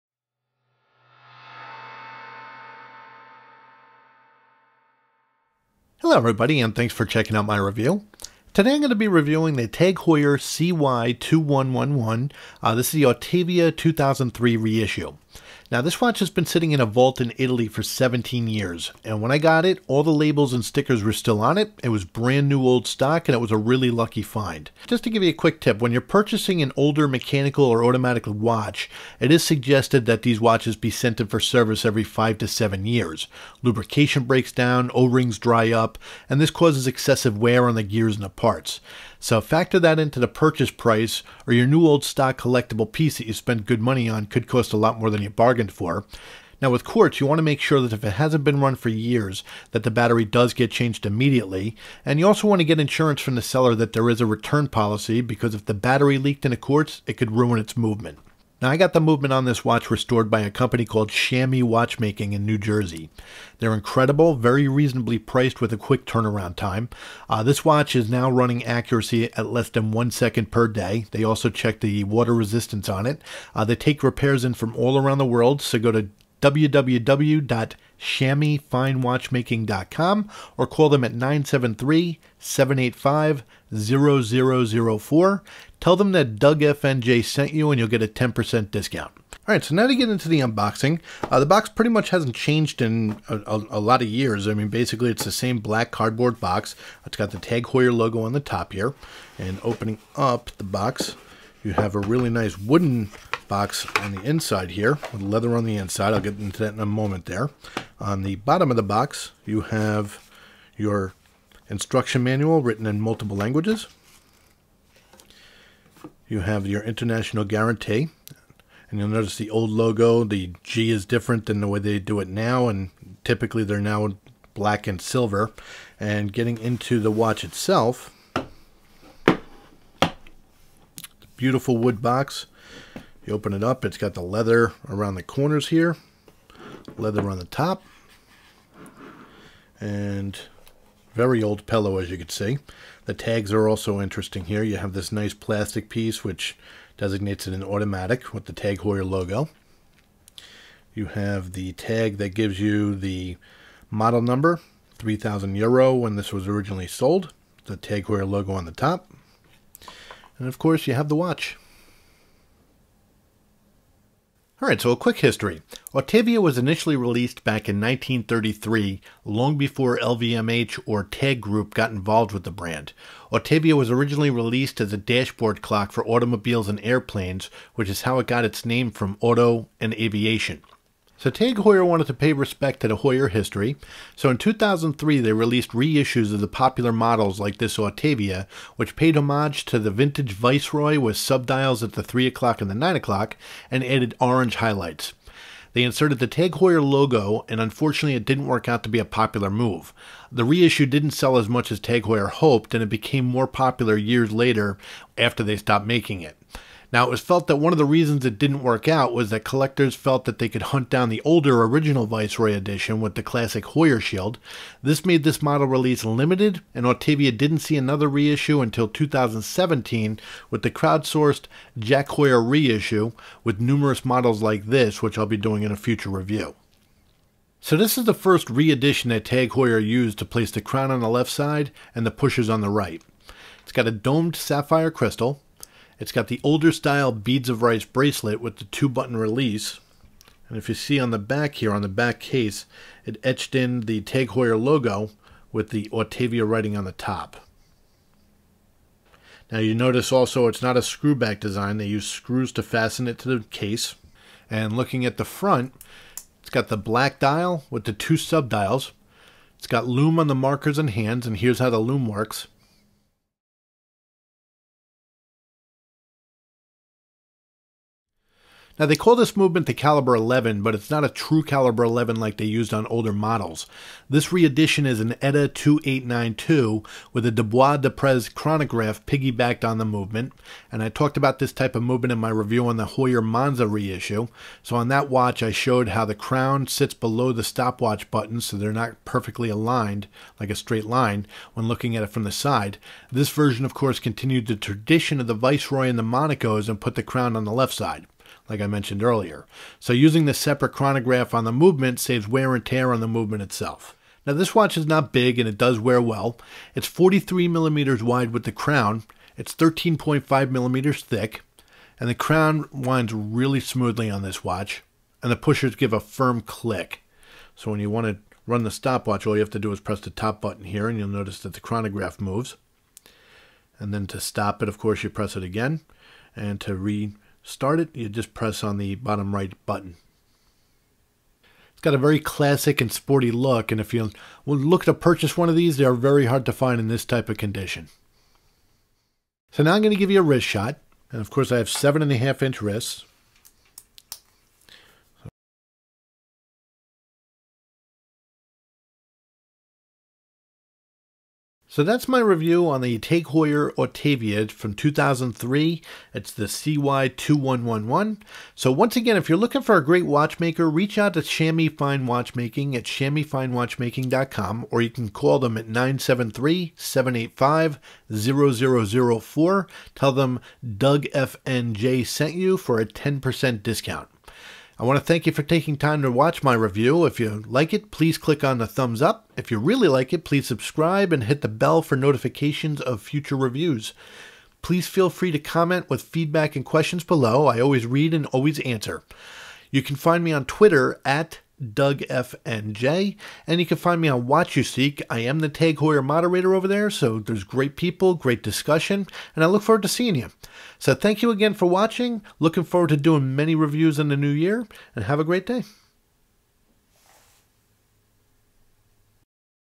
Tag Heuer Autavia 2003 Reissue CY2111 with modified Calibre 11 Movement.
Hello everybody, this video is of a 17 year old watch I received from Italy.  It was sitting in a vault unworn and arrived with stickers, box, tags, and paperwork.Having been sitting for so long, seals and O-Rings dry up, lubrication breaks down.  I brought it to a Watchmaker, and they serviced the watch inside to factory new condition and running less than 1 second per day accuracy.  Enjoy this short review on the watch and its history.